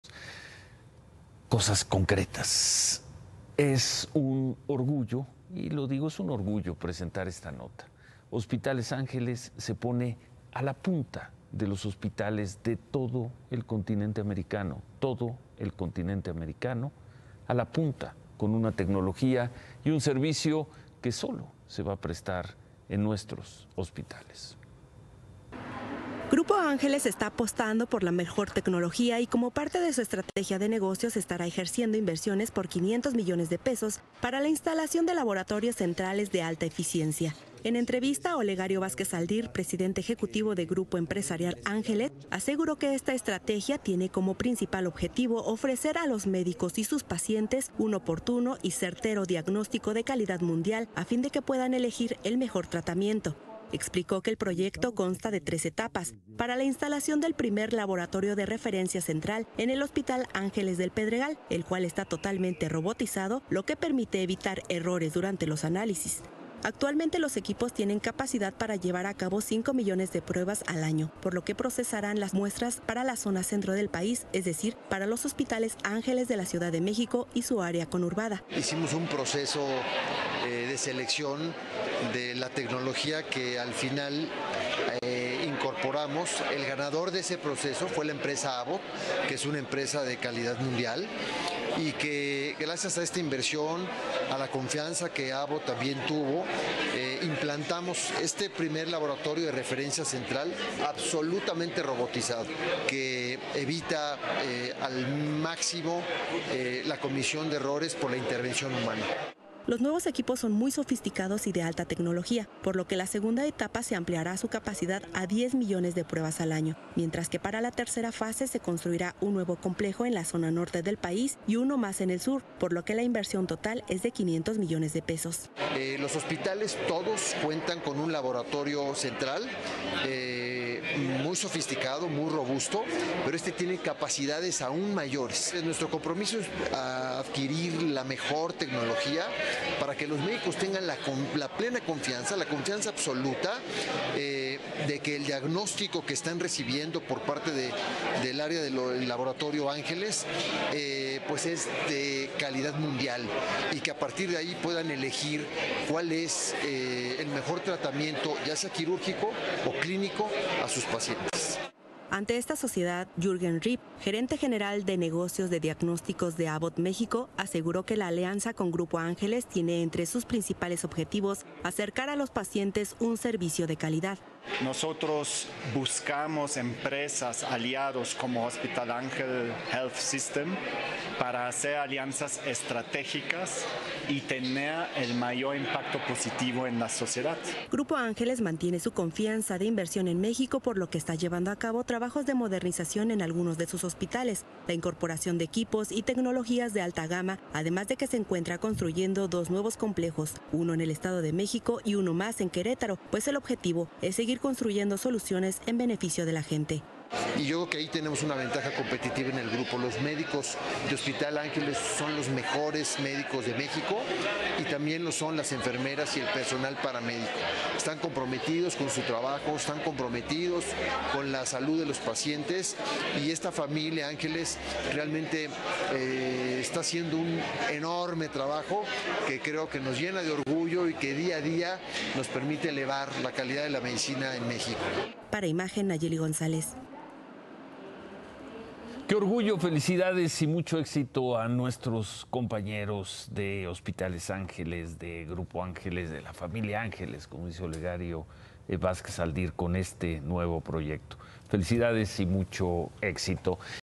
Grupo Empresarial Angeles está apostando por la mejor tecnología | Ciro Gómez Leyva
CiroEnImagen El presidente ejecutivo de Grupo Empresarial Angeles, OlegarioVázquezAldir, informó que GrupoAngeles está apostando por la mejor tecnología y como parte de su estrategia de negocio se estará ejerciendo inversiones por 500 mdp para la instalación de laboratorios centrales de alta eficiencia.